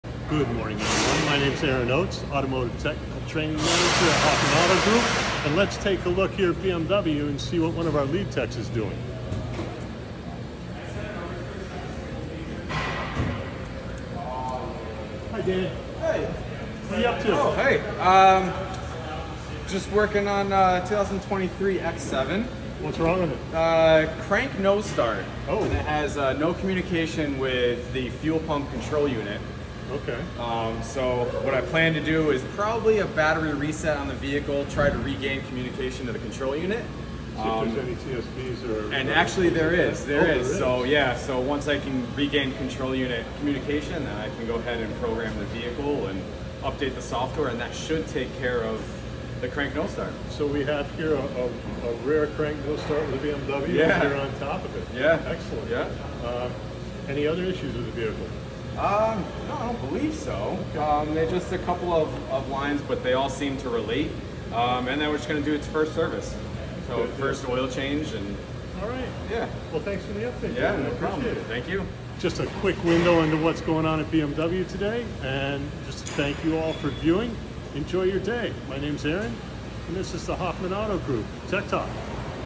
Tech Talk Episode 36: Let's see what Master Technician Dan is up to!
Aaron checks in on Dan, Master Technician at BMW of Waterbury!